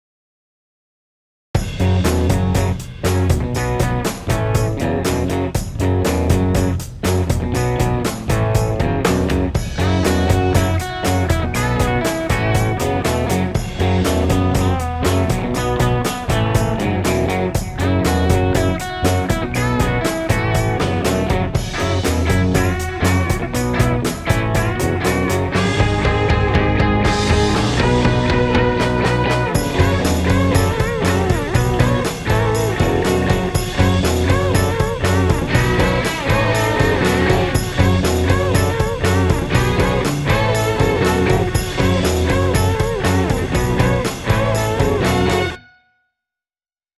Pinko Up Spring/Summer 2015 Collection | Childrensalon
With a combination of colourful casualwear and sporty styles, enjoy the latest Pinko Up collection, shown in this playful behind the scenes video.